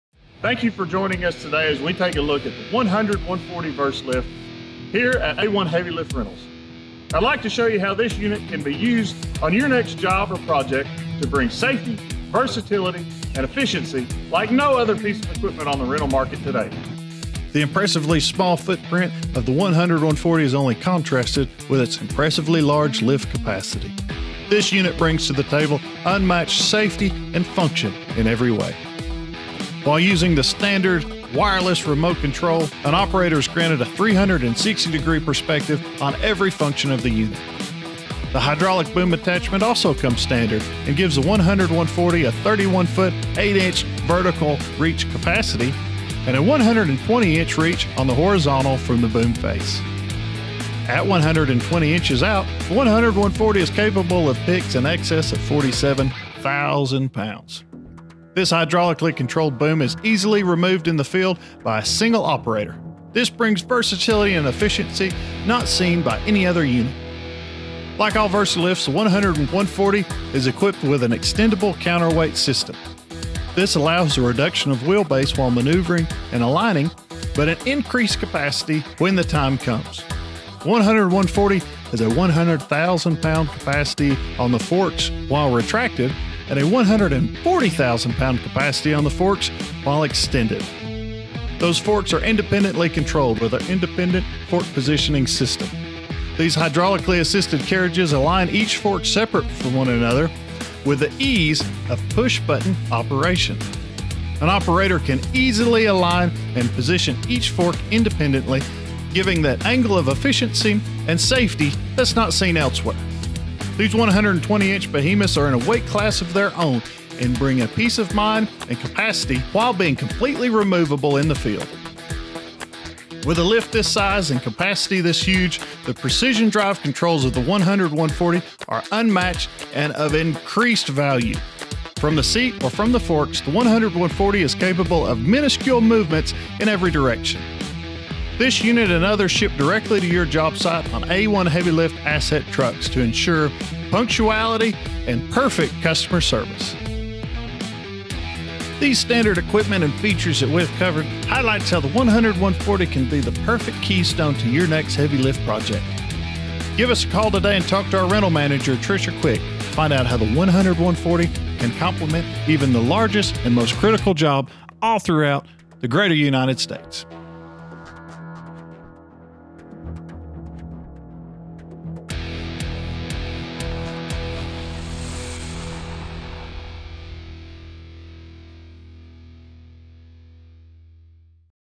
Spotlight: 100/140 Versa-Lift
The key features and functions of the commanding 100/140 Versa-Lift from A1 Hevi-Lift Rentals are covered and discussed. Learn how the 100/140 Versa-Lift can add Safety, Versatility, and Efficiency to your next heavy-lift project!